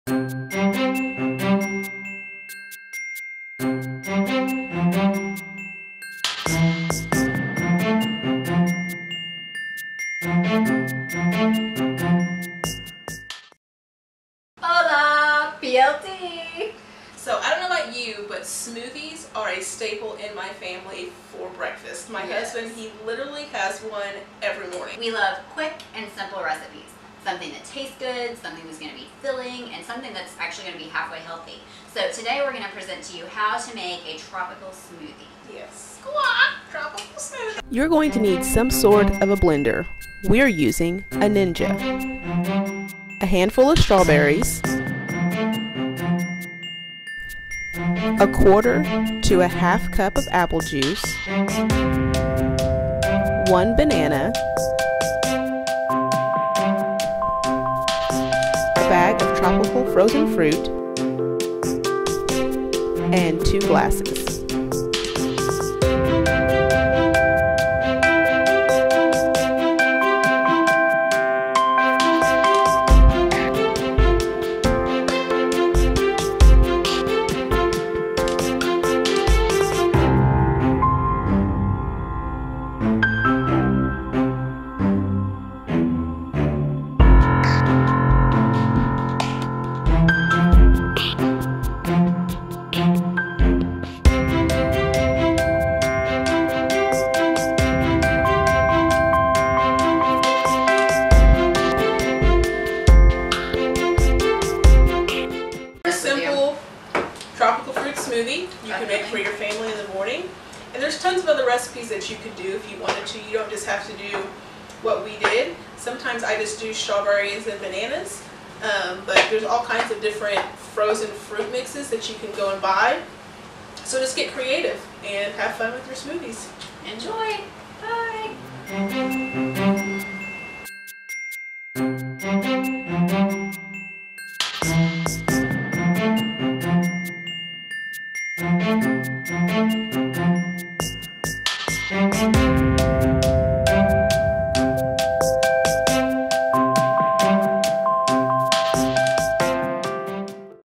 Quick & Simple Tropical Smoothie Recipe
We show you how to make a quick and simple tropical smoothie. This is great for breakfast or a midday snack. The kiddos love them too!
We also give you the calorie content at the end of the video!